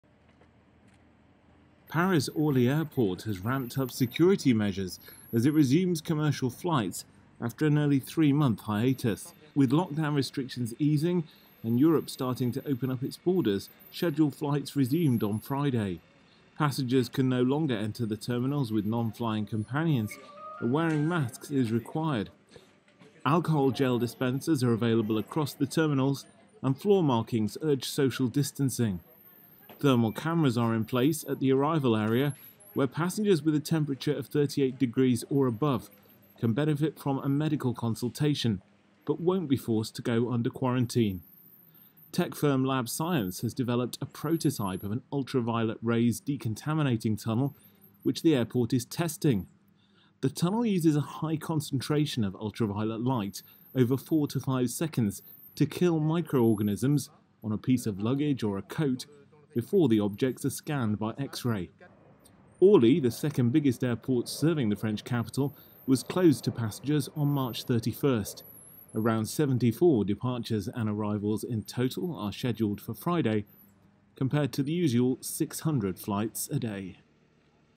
Paris airport's 'new normal' comes with UV tunnel, thermal cameras
Paris's Orly Airport has ramped up security measures as it prepares to resume commercial flights after a nearly three-month hiatus.